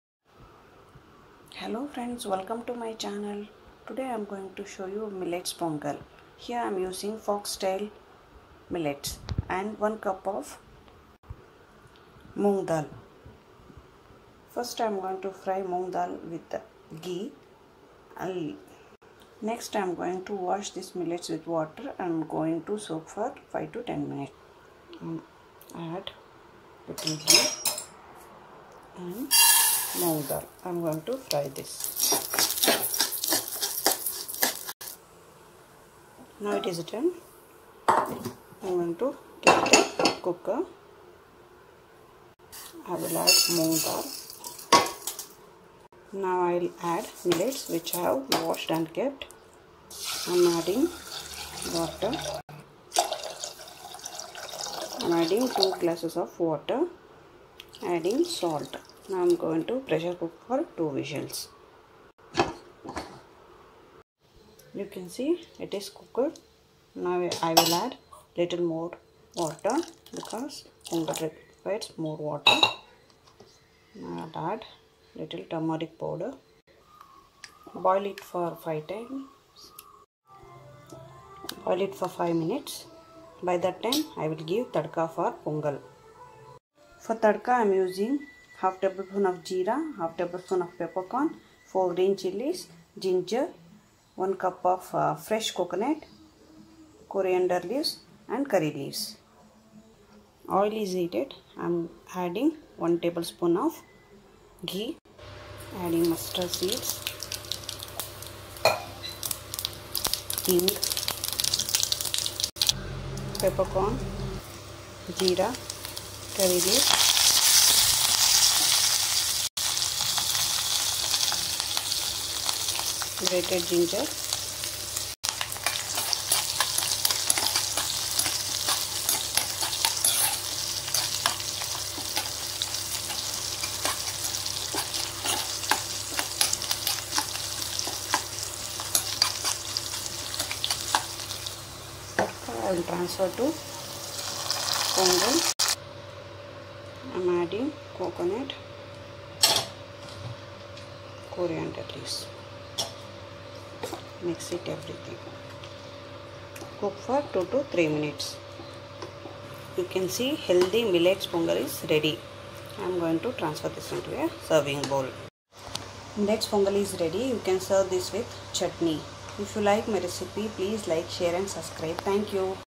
foxtail pongal millets
Hello everyone wel;come  to my channel saviruchi by savi
Ingridents:
 one bowl foxtail
one cup moong da;
peppercorn
jeera
ghee and oil
curryleaves
mustard seeds
corriander leaves
grated ginger
Hing